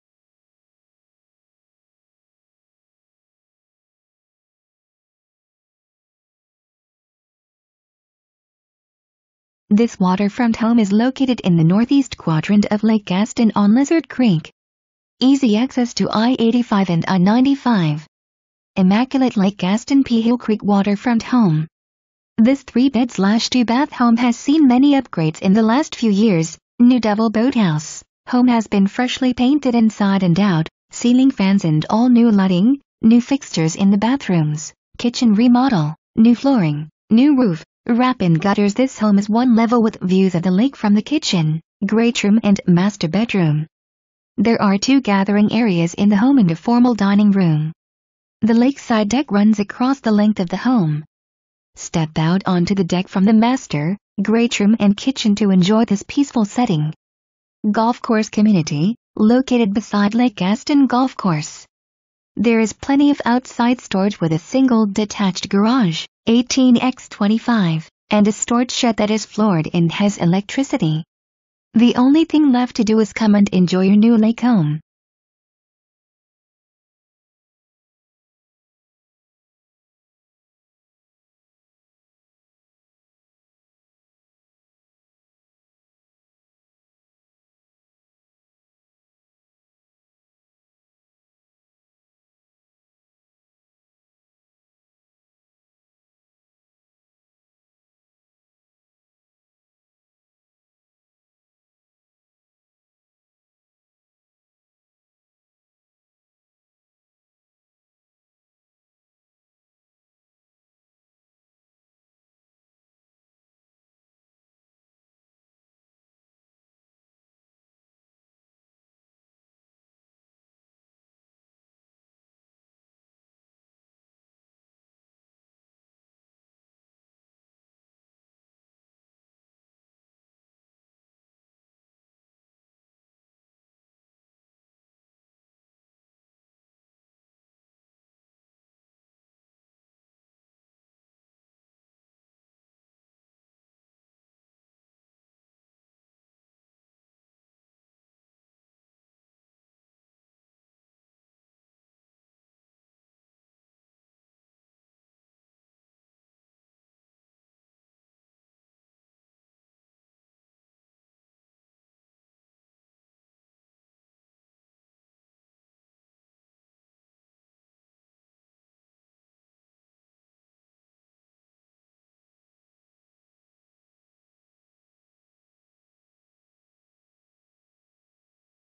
Homes for Sale - 680 Country Club Dr, Gasburg, NC
Immaculate Lake Gaston Pea Hill Creek waterfront home . This 3 bd/ 2 ba home has seen many upgrades in the last few years, New Double Boathouse, home has been freshly painted inside and out, ceiling fans & all new lighting, new fixtures in the bathrooms, kitchen remodel, new flooring, new roof, wrap and gutters.. This home is one level with views of the lake from the kitchen, greatroom and master bedroom. There are two gathering areas in the home and a formal dining room. The Lakeside deck runs across the length of the home. Step out onto the deck from the Master, Greatroom and Kitchen to enjoy this peaceful setting. Golf Course community, located beside Lake Gaston Golf Course. There is plenty of outside storage with a single detached garage (18 X 25) and a storage shed that is floored and has electricity. The only thing left to do is come and enjoy your new Lake home .

Presented By:
Brenda Thompson, Coldwell Banker Advantage